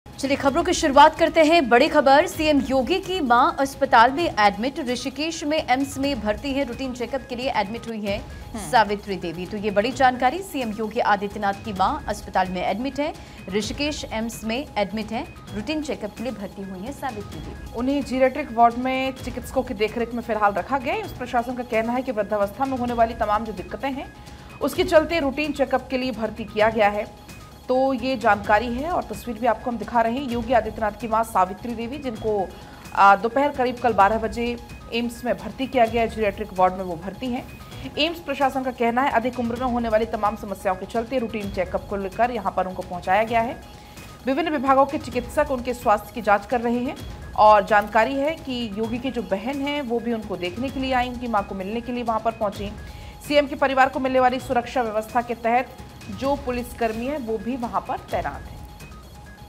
सीएम योगी की मां सावित्री देवी अस्पताल में भर्ती | AIIMS Hospital | Rishikesh | UP | Breaking News
Anchor Name : Ranjana Rawat | Show Name :News | Time : 08: 00 AM | Date : 15/05/2024 | Published By:Vanshika